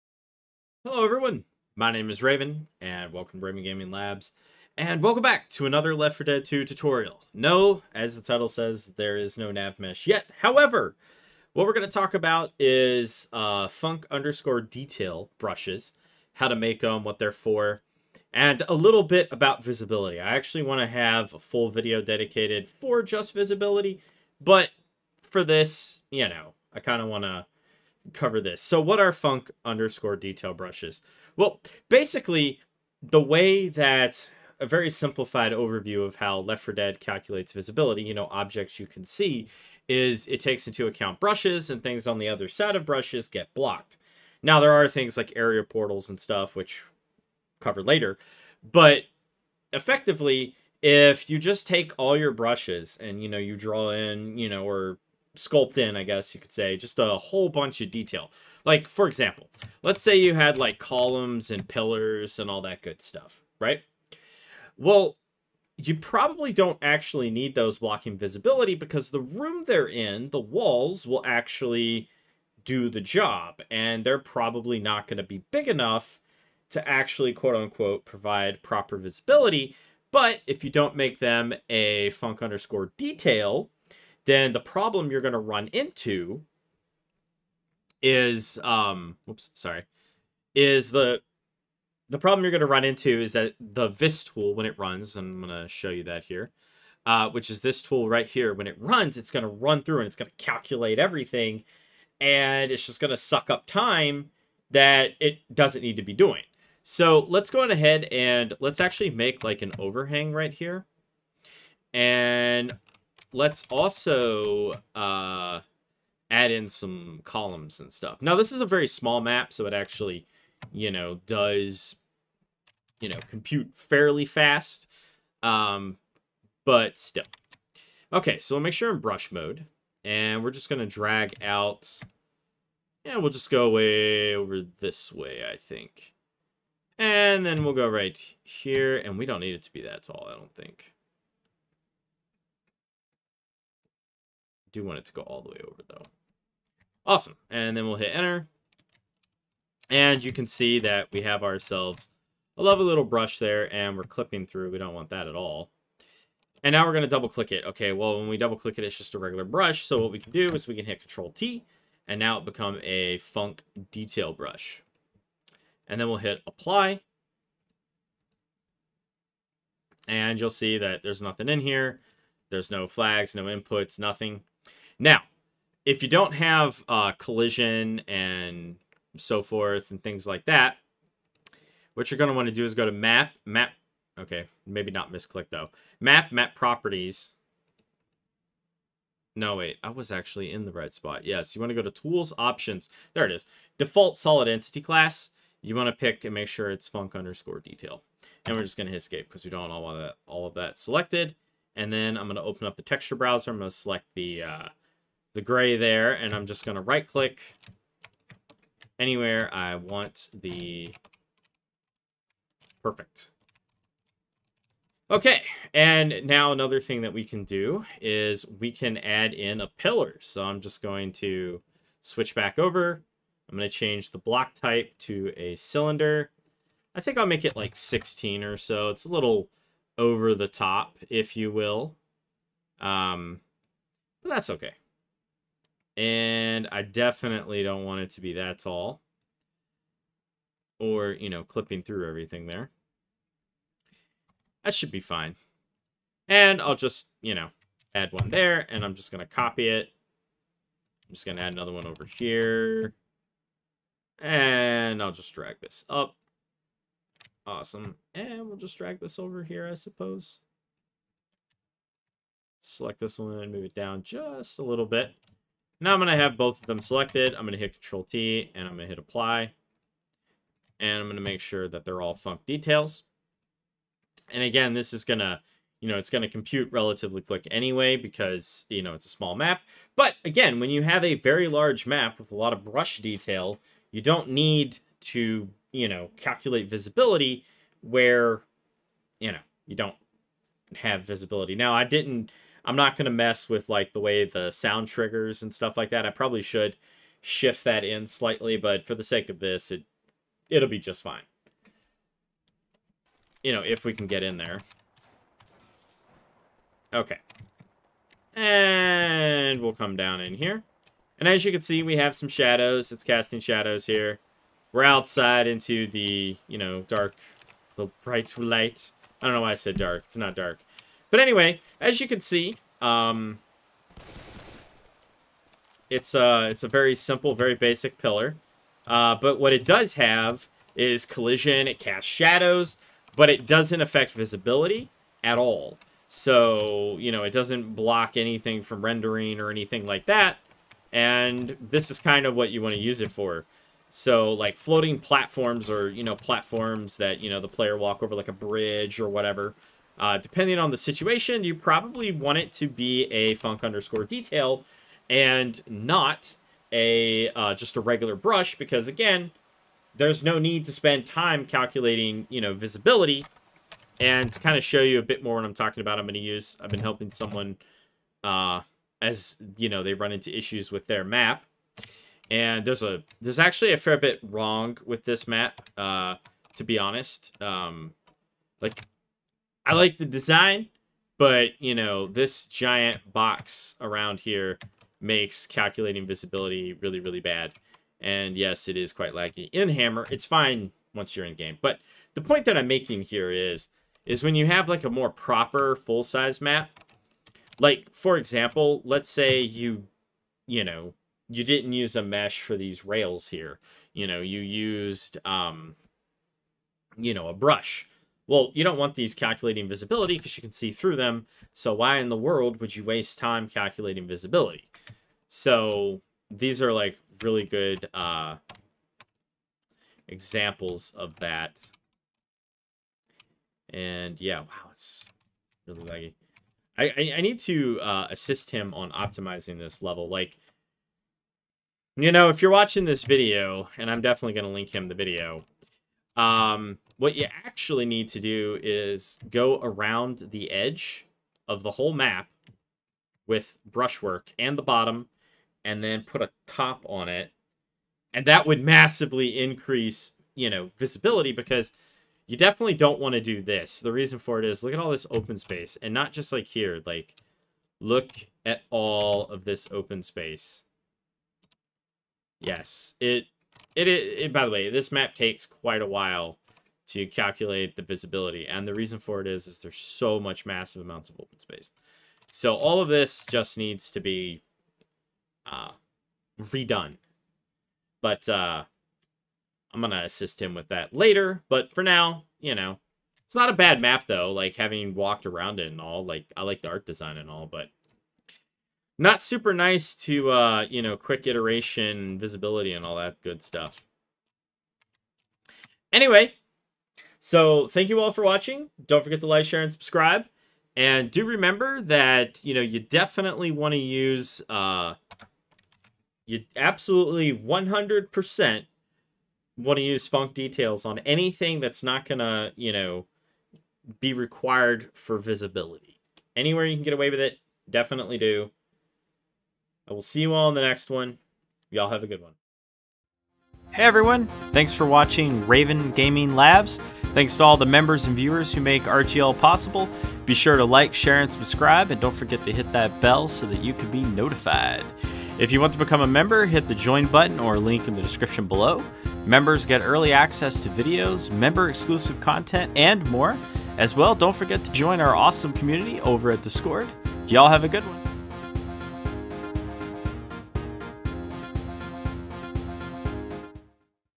Left 4 Dead 2 SDK Mapping Tutorial - Func_Detail and Visibility - Tutorial 28 - 2023
In this Left 4 Dead 2 tutorial we will look at using Func_Detail as a way to optimize vis build times for brushes that won't add block visibility anyway.

*** Chapters ***
00:00 - Intro
00:27 - Func_Detail
06:03 - Bad Visibility Maps
08:25 - Outro